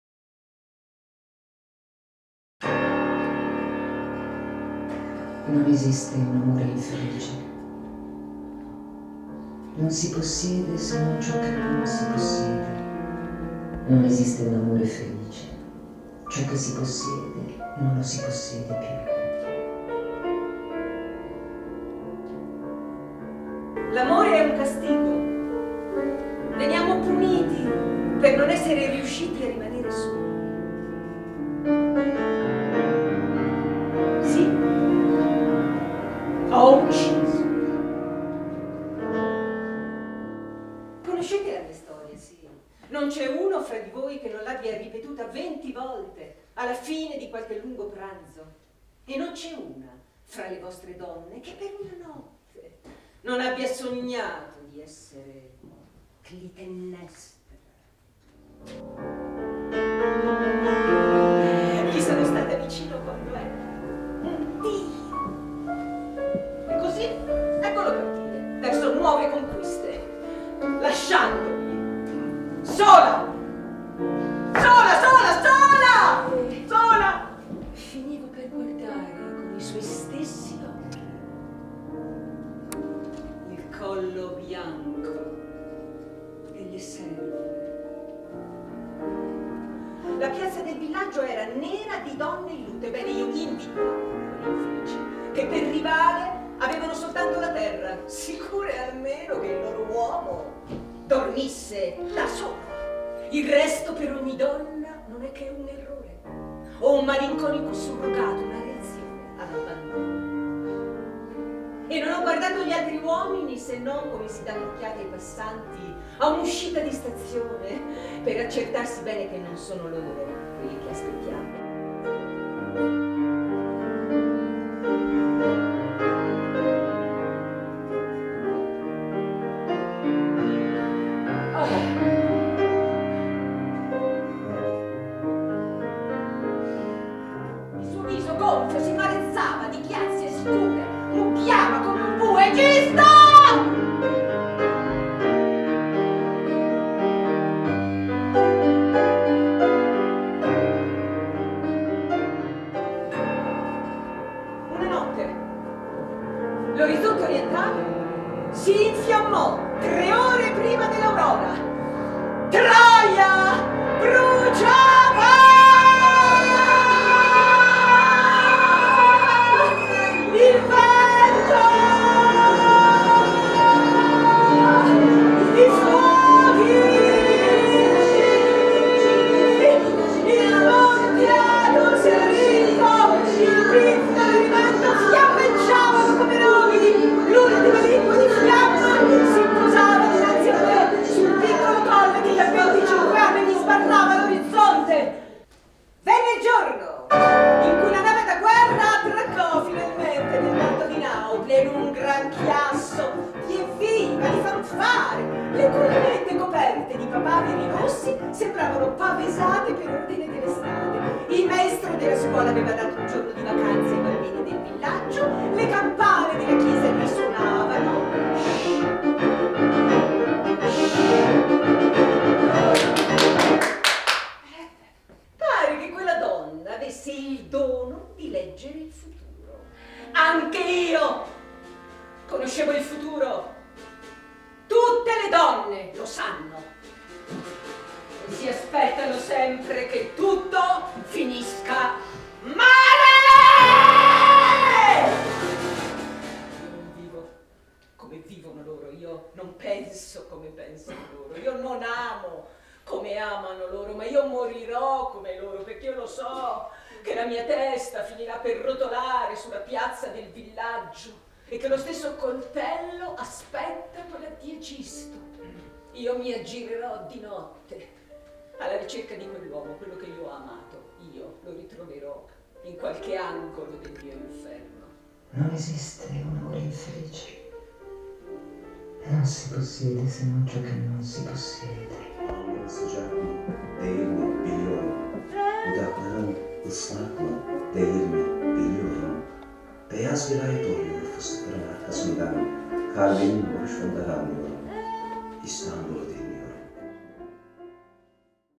Clitennestra di Marguerite Yourcenar
Clitennestra
di Marguerite Yourcenar
regia scenica e realizzazione video Maria Luisa Bigai
con Anita Bartolucci
e al pianoforte Danilo Blaiotta su musiche di Alessandro Molinari
la poesia in turco è letta da Omar As
disegno luci Marco Catalucci
elementi di scena e progetto costume Dora Argento
produzione Musamoi  Associazione Culturale

Lo spettacolo è ambientato tra i fumi di un Caffè di Istanbul, degli anni '20, mentre un pianista strimpella e lo spettacolino deve avere inizio, sullo scorcio di una Turchia che è una soglia fra epoche e dimensioni diverse. Una Donna (la chanteuse del caffè?) dice i suoi perché e affonda con lucidità e passione nel proprio crimine (una memoria ossessiva? un'affabulazione? una prova in camerino?). La stessa storia ha sapori molto diversi nella bocca di chi la racconta... La Clitennestra della Yourcenar è una Clitennestra che ha ucciso per amore e sa quasi riderne, a tratti, mentre racconta  (o ricorda? o reinventa?) un Agamennone imbolsito e tronfio che non sa più vedere; un Egisto giovinetto che non potrebbe colmare nè consolare mai il dislivello della passione. Clitennestra o una Donna che cerca ancora e infinitamente lo sguardo dell'amato, ombra sfinita fra le pareti della memoria, fra i meandri del mito, eternata e dannata, infinitamente.  Alessandro Molinari ha composto per lo spettacolo musiche originali, che esegue al pianoforte dal vivo, come se fosse il pianista del caffè, che cerca sulla tastiera i suoi refrains, oppure il pianista da cinema muto che accompagna una vicenda "fantasmica come un film"...  D'altronde quale migliore evocazione dei fantasmi del mito, negli anni '20, che le facce in bianco e nero che appaiono mute sullo schermo?  E dopotutto questa Clitennestra è anche una donna che non s'è resa conto del passare degli anni. E non lo accetta. Come certe Dive del Cinema e come molte donne assai meno famose.  E si condanna a una eterna sospensione, a un'eterna attesa. Come è l'attesa in camerino, la pausa fra un numero e l'altro, la pausa in attesa di un appuntamento d'amore che si continua a sperare che arrivi, o ritorni.  Vi sono quindi interventi video che danno corpo nello spettacolo a fantasmi e memorie che riappaiono e che permangono nel tempo e nell'attesa.  Vi sono echi registrati della voce di Clitennestra, come il risuonare della memoria quando si dipana, come la ripetizione a sbloccare un meccanismo che si inceppa, o l'ingigantirsi di un pensiero, come il permanere delle voci nelle stanze del tempo.